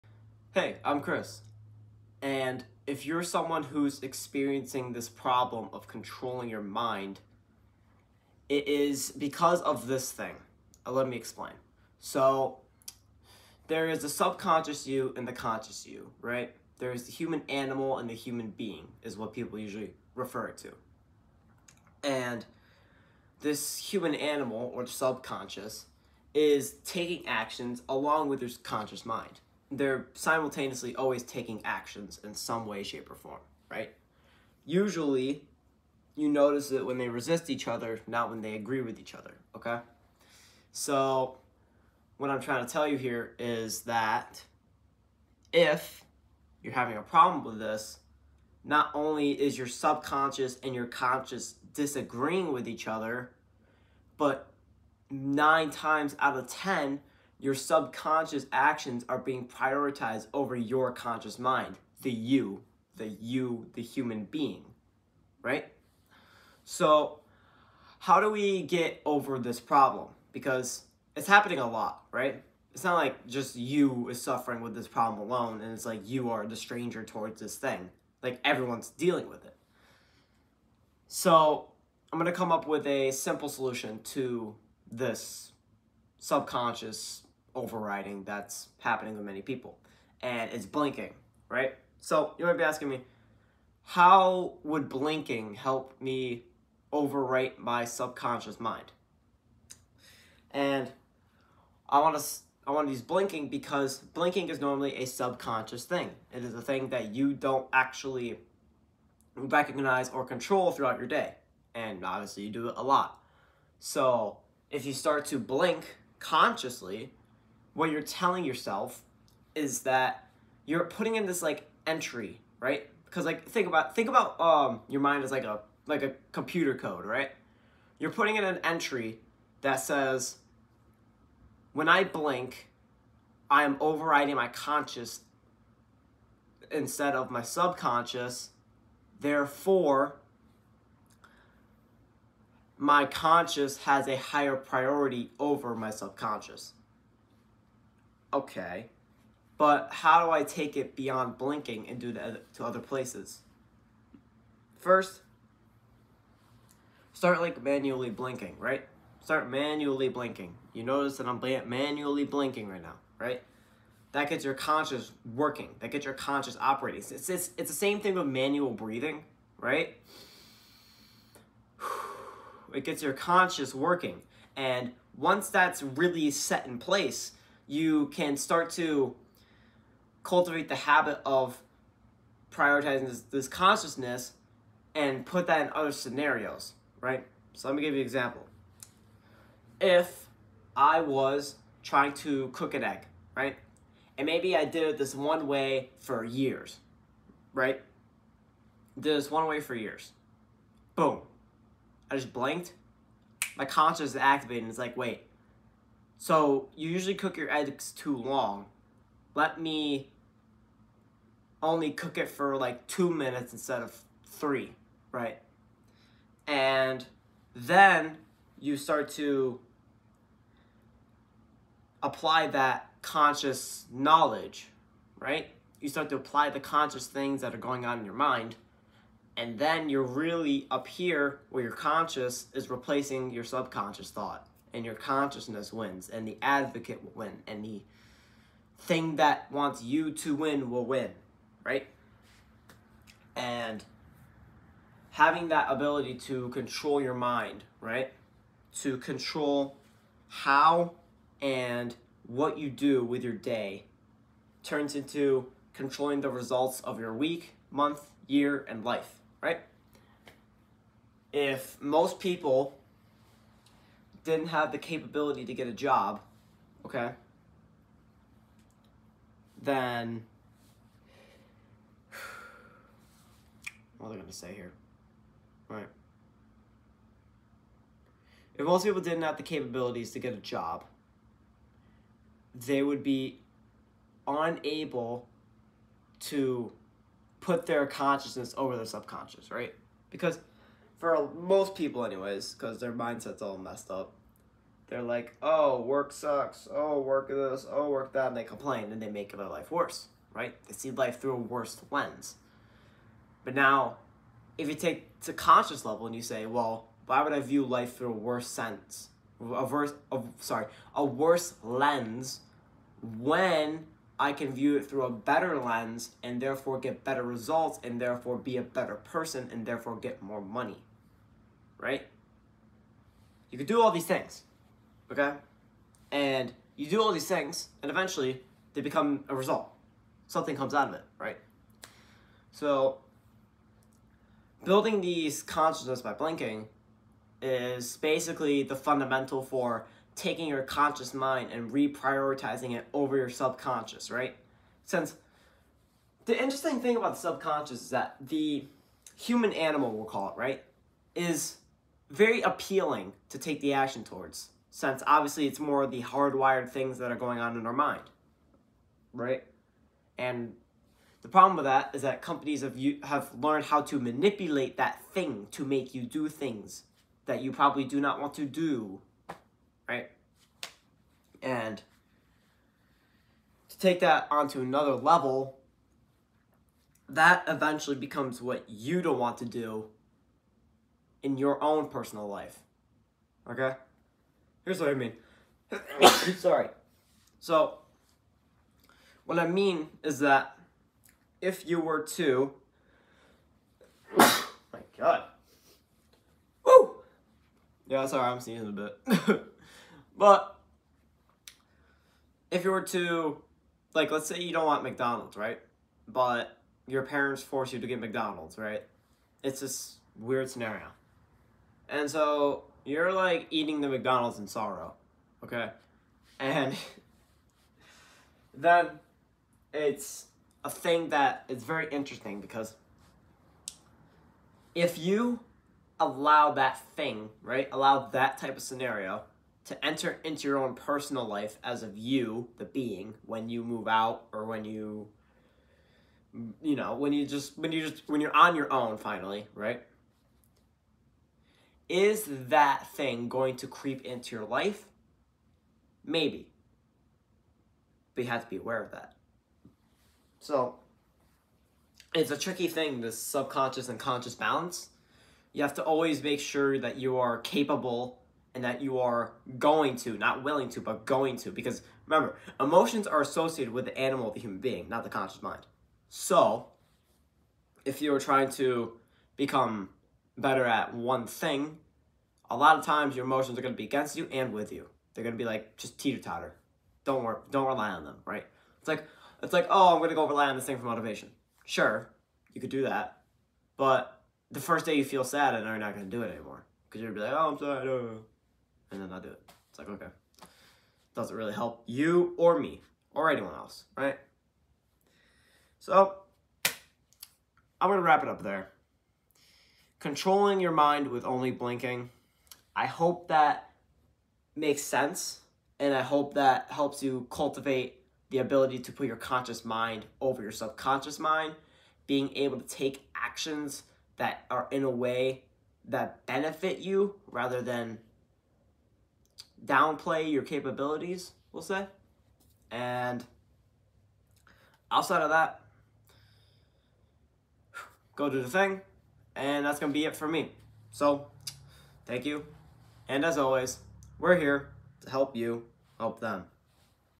Control Your Mind by Just Blinking (Must WATCH!)
This about using simple conscious intervention to deprioritize the subconscious actions of the human animal, and putting the human being at the forefront of all actions in your life.